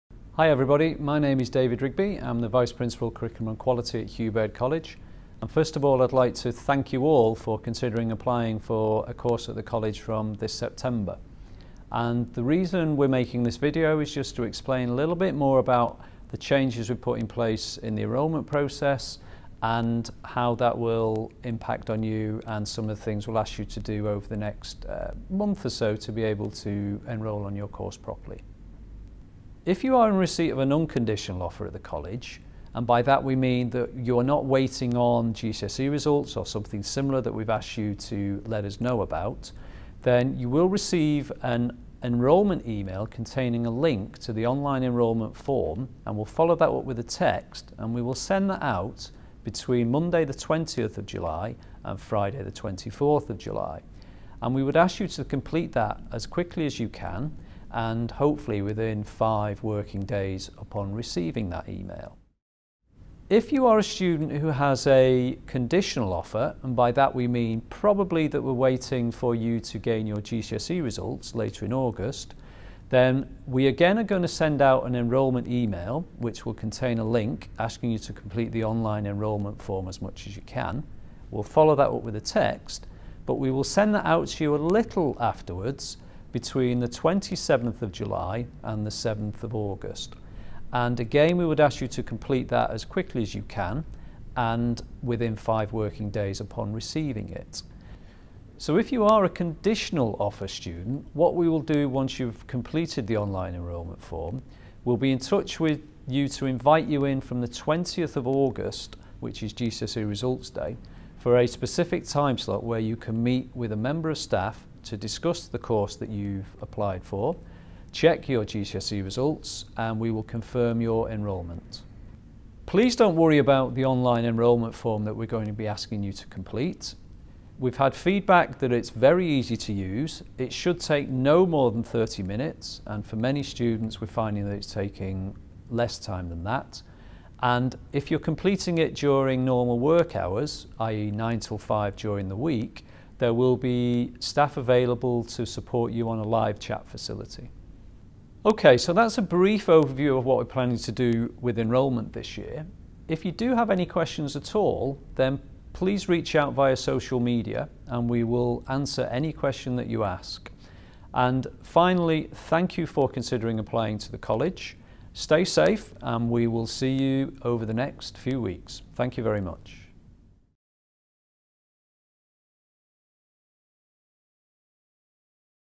College Enrolment 2020
If you have an offer to join the College this September, please watch this video which explains our enrolment process this year.

Note: this video is for those with offers to join College (excluding; 14-16 College, A-levels, Nurse Cadets, Learning for Life, Apprenticeships and University Centre).